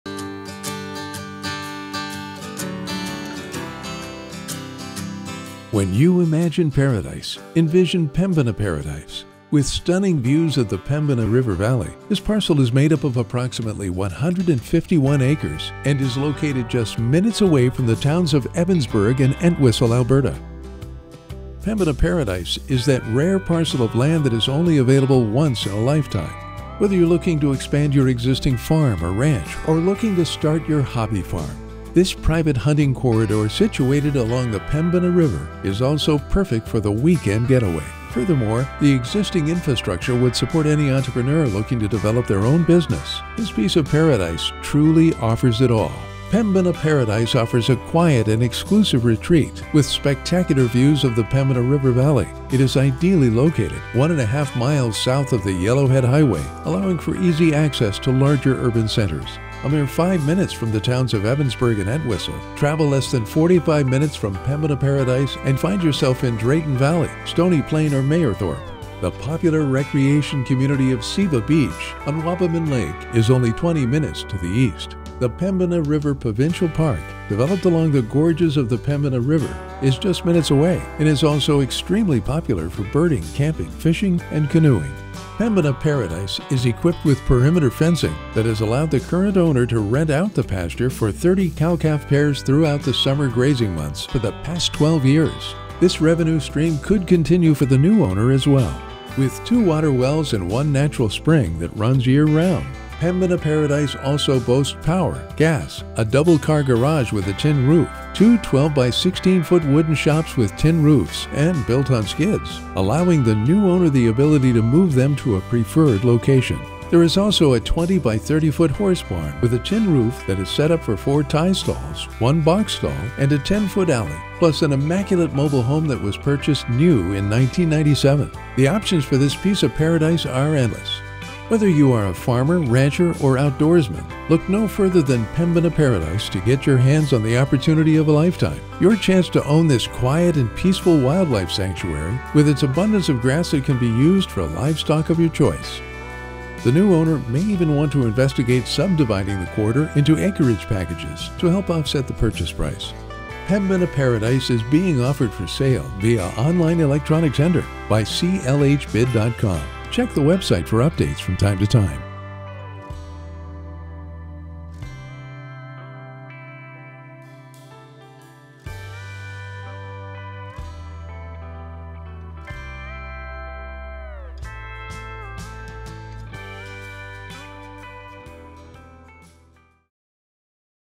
CLHbid.com - Pembina Paradise
Pembina Paradise For Sale - AB

Selling on Tuesday, November 29, 2022 at 9:00 AM MST

Over 150 acres for sale just 1.5 miles south of Evansburg, Alberta.  With spectacular views of the Pembina River Valley, this quiet oasis offers unlimited opportunities.  Whether looking for ranch land, farm land, a private hunting retreat or a secluded haven just minutes away from the Pembina River Provincial Park or Wabamun Lake, look no further.